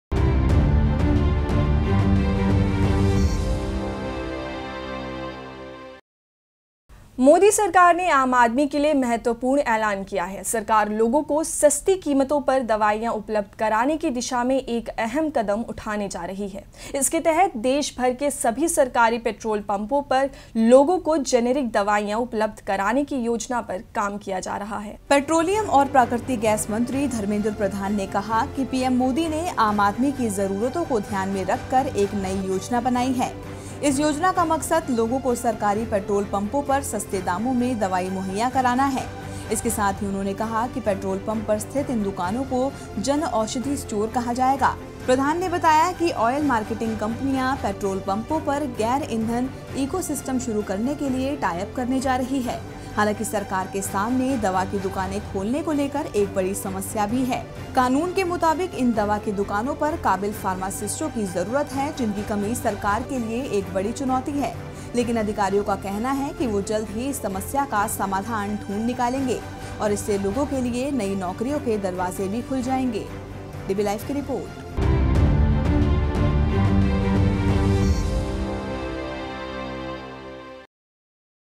पेट्रोल पंपों पर अब मिलेगी दवा | Government plans to sell generic medicines at fuel stations
मोदी सरकार ने आम आदमी के लिए महत्तवपूर्ण ऐलान किया है। सरकार लोगों को सस्ती कीमतों पर दवाईयां उपलब्ध कराने की दिशा में एक अहम कदम उठाने जा रही है। इसके तहत देश भर के सभी सरकारी पेट्रोल पंपों पर लोगों को जेनेरिक दवाईयां उपलब्ध कराने की योजना पर काम किया जा रहा है।
पेट्रोलियम और प्राकृतिक गैस मंत्री धर्मेंद्र प्रधान ने कहा कि पीएम मोदी ने आम आदमी की ज़रूरतों को धयान में रखकर एक नई योजना बनाई है। इस योजना का मकसद लोगों को सरकारी पेट्रोल पंपों पर सस्ते दामों में दवाई मुहैया कराना है। इसके साथ ही उन्होंने कहा कि…पेट्रोल पंप पर स्थित इन दुकानों को ‘जन औषधि स्टोर’ कहा जाएगा। प्रधान ने बताया कि…ऑयल मार्केटिंग कंपनियां पेट्रोल पंपों पर गैर-ईंधन इकोसिस्टम शुरू करने के लिए टाईअप करने जा रही हैं। हालांकि, सरकार के सामने दवा की दुकानें खोलने को लेकर एक बड़ी समस्या भी है। कानून के मुताबिक इन दवा की दुकानों पर काबिल फर्मासिस्टों की ज़रूरत है जिनकी कमी सरकार के लिए एक बड़ी चुनौती है। लेकिन अधिकारियों का कहना हैं कि वो जल्द ही इस समस्या का समाधान ढूंढ निकालेंगे और इससे लोगों के लिए नई नौकरियों के दरवाज़े भी खुल जाएंगे।